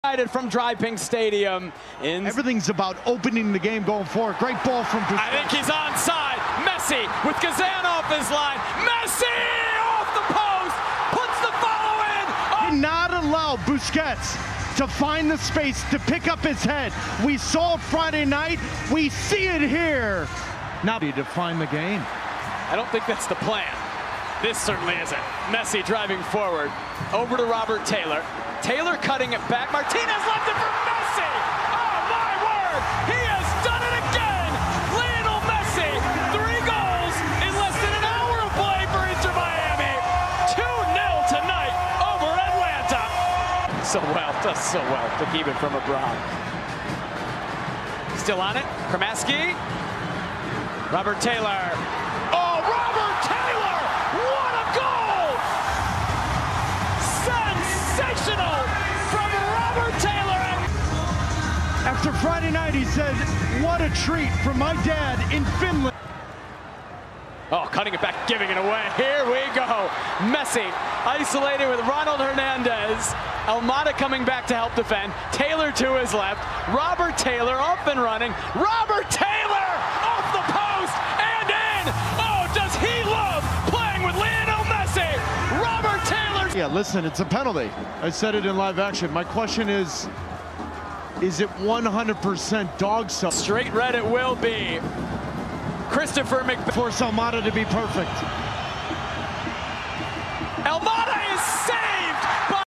Messi With 2 Goals! Inter Miami CF vs. Atlanta United | 4-0 GOALS HIGHLIGHTS
Messi scored 2 goals against Atlanta United and Secured Inter Miami 3 points.